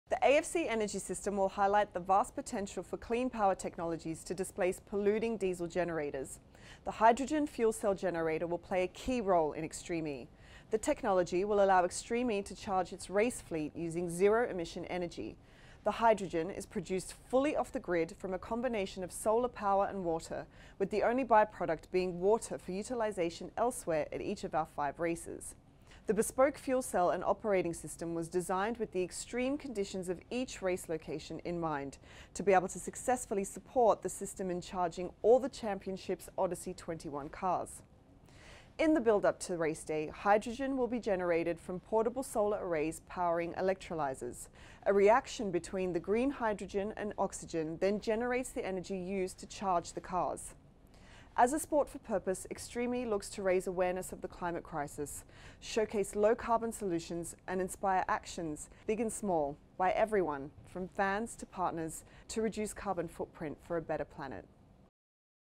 Izy on AFC Energy's Hydrogen Fuel Cell
Extreme E's Izy Rekiel explains how the AFC Energy fuel cell will be used to power the Extreme E rally series